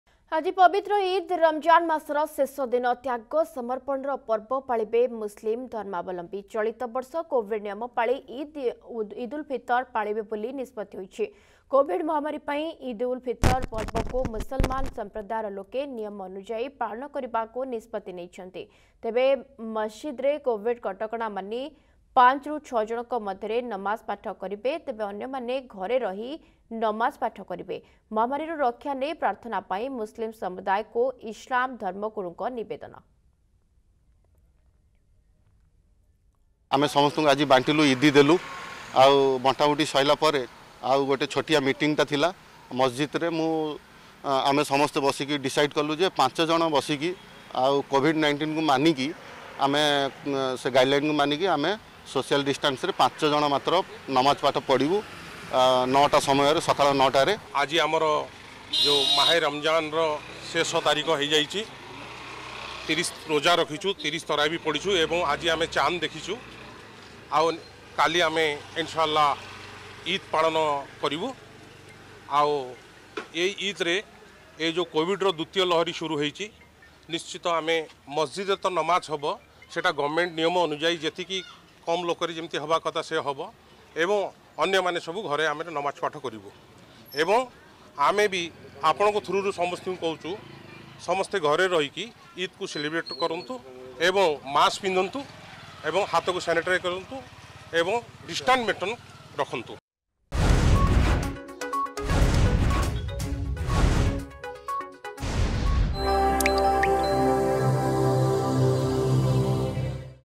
ଆଜି ପବିତ୍ର ଇଦ ...
ଆଜି ପବିତ୍ର ଇଦ ।  ରମଜାନ ମାସର ଶେଷ ଦିନ । ତ୍ୟାଗ ସମର୍ପଣର ପର୍ବ ପାଳିବେ ମୁସଲିମ ଧର୍ମାବଲମ୍ବୀ। ଚଳିତ ବର୍ଷ କୋଭିଡ ନିୟମ ପାଳି ଇଦ ଉଲ ଫିତର ପାଳିବେ ବୋଲି ନିଷ୍ପତ୍ତି ହୋଇଛି ...